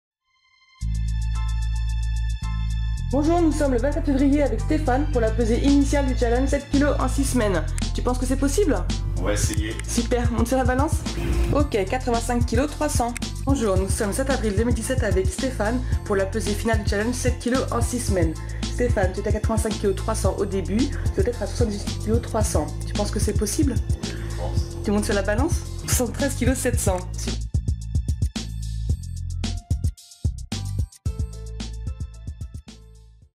Transformation physique rapide Rennes
Stéphane a perdu 11,600 kilos en 6 semaines à la Training Academy de Rennes.